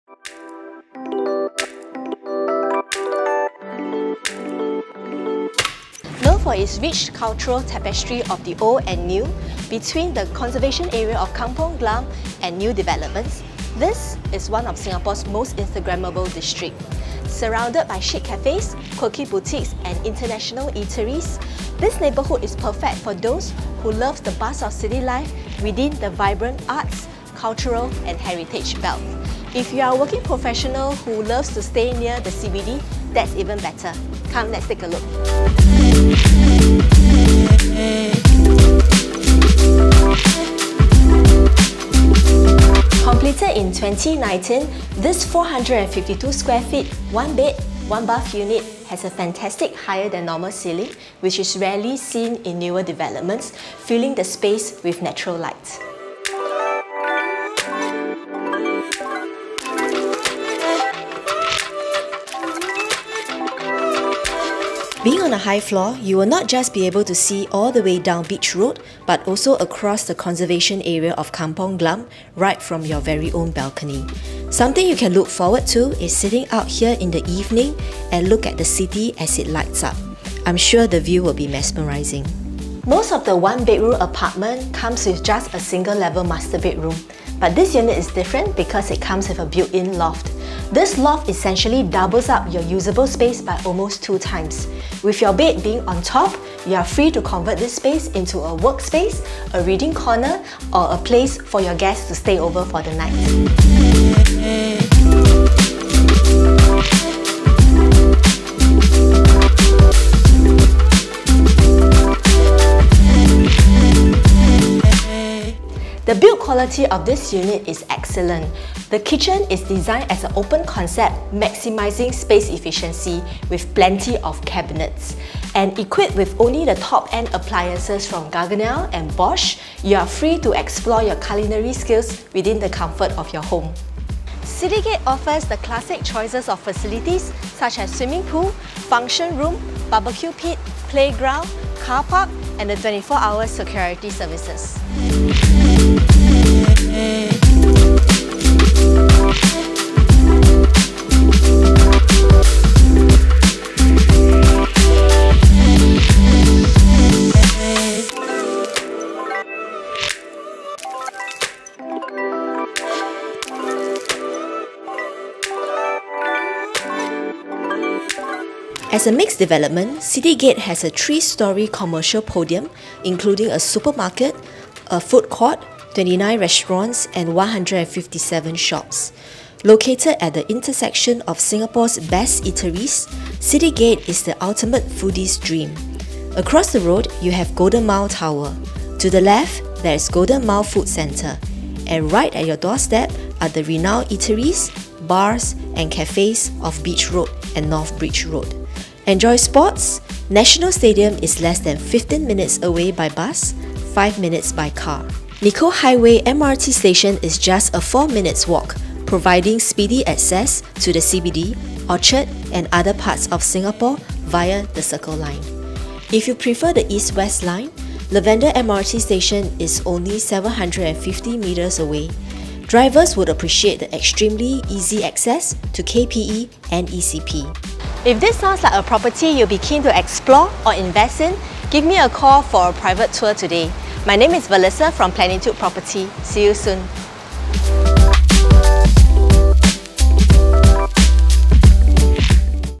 City Gate 1 bed 1 bath with mesmerising views For SALE
[EXCLUSIVE LISTING By Plenitude Property]

Anyone looking to stay in the heart of town surrounded by chic cafes, exciting activities, shopping and great food?
This fabulous unit is on a super high floor, allowing u to enjoy panoramic view of the city’s sky scrapers right from your balcony.

A much higher ceiling than the normal, the master bedroom already comes with a built in loft which provides you extra square footage for a comfortable living.

The build quality is excellent. Its kitchen is equipped with top end appliances from Gaggenau and Bosch.

This neighbourhood is known as a rich cultural tapestry of the old and new making it one of Singapore’s most Instagrammable district. Arab street is just across the road where u will be spoilt for choices when it comes to food and drinks choices.

Nicoll Highway is just 4 min walk away which will link u to all the other major MRT lines.

For those who love the buzz of city life yet located within the vibrant arts, culture and heritage belt or for working professionals who want to live close to the CBD, this unit will be a delightful one.

1 bed • 1 bath • 452sqft
Exclusively listed by Velisa Leong